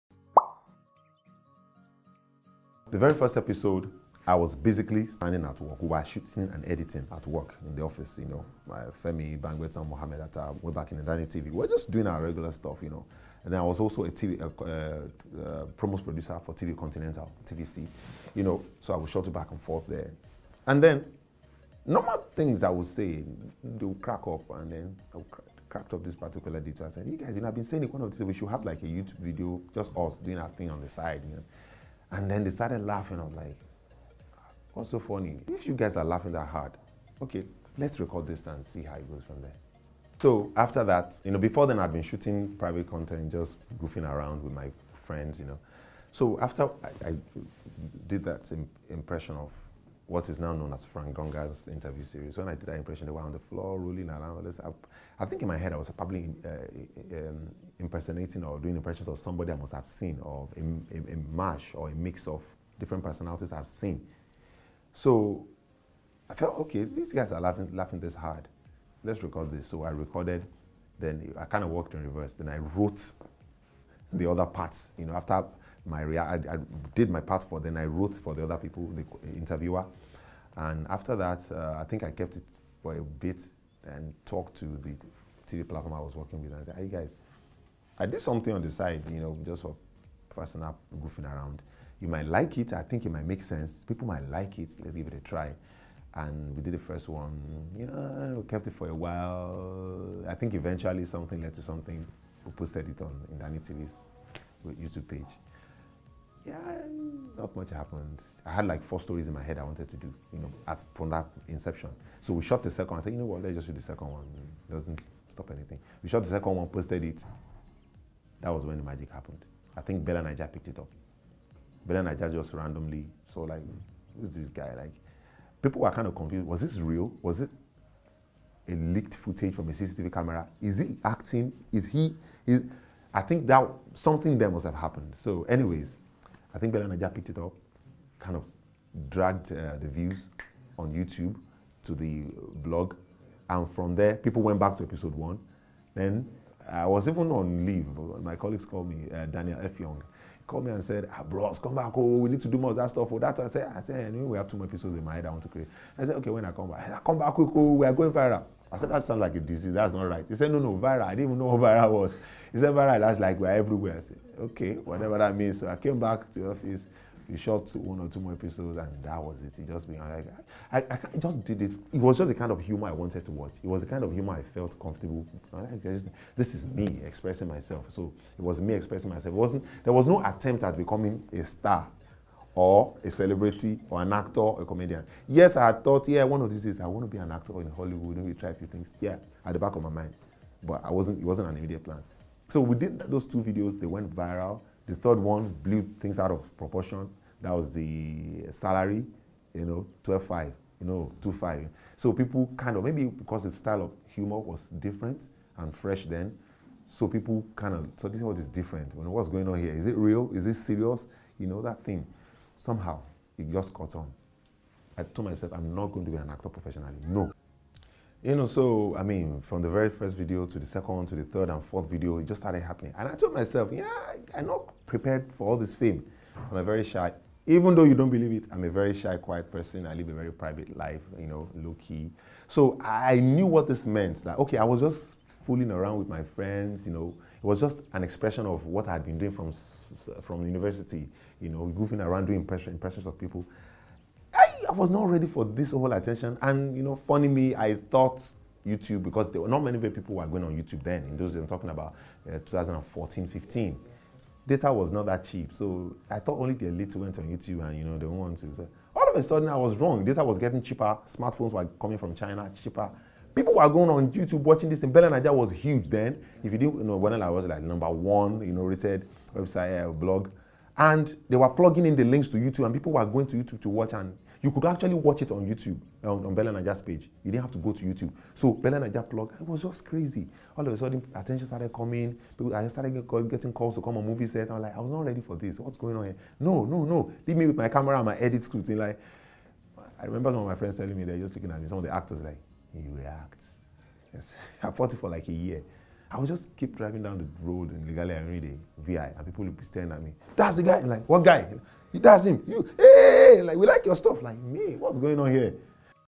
Frank Donga reveals how to create your first viral skit
In this interview with Isaac Oladipupo, Frank Donga shares his journey like never before while revealing what makes him tick.

Kunle Idowu, popularly known as Frank Donga, is a Nigerian actor and comedian. He rose to fame through the webseries, The Interview, on Ndani TV about an unassuming job-seeker for which he was nominated for Best Actor in a Comedy in the Africa Magic Viewers Choice Award in 2015.